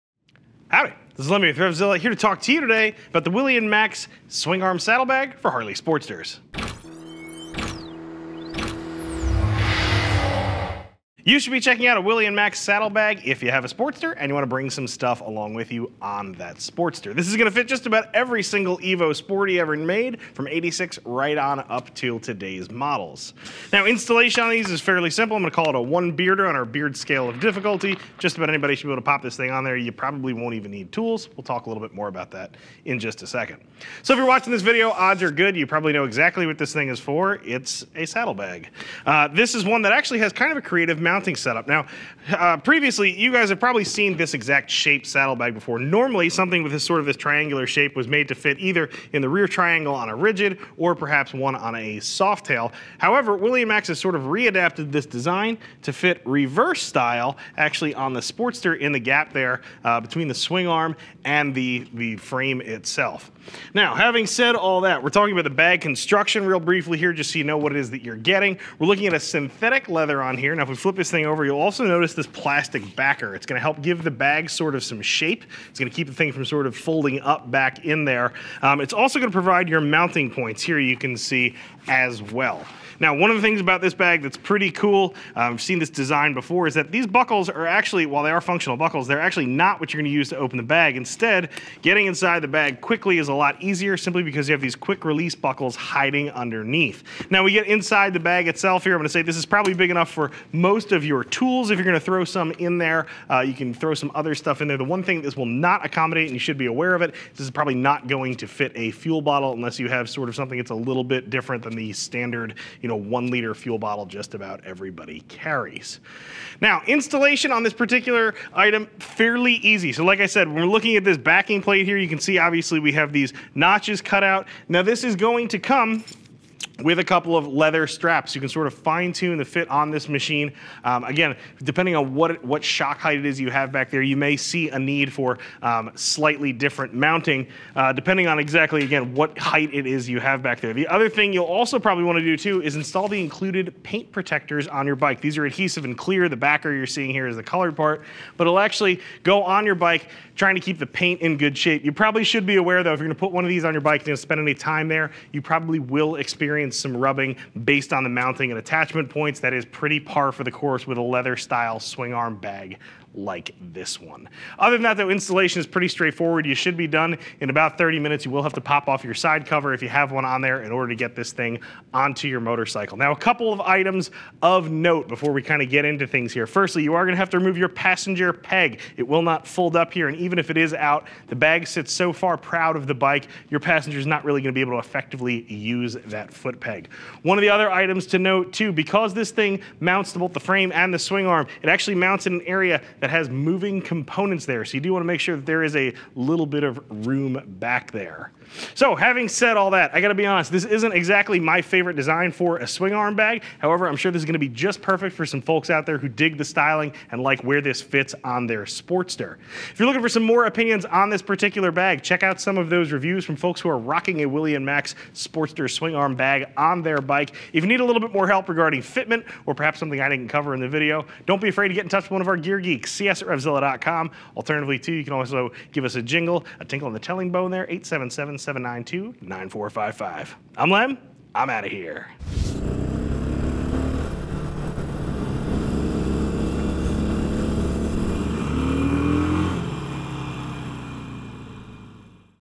Willie & Max Swing Arm Saddlebag for Sportster Review
Tuck some cargo in an unused part of your Sporty with the Willie & Max Swing Arm Saddlebag. These synthetic leather saddlebags are durable, secure, and available in several colors to match your motorcycle. Quick-release buckles hidden under the flap will allow easy access to the motorcycle while retaining an old-school look. Use the included straps to fine-tune the mounting on your Sportster. Don’t forget the paint protectors, also included!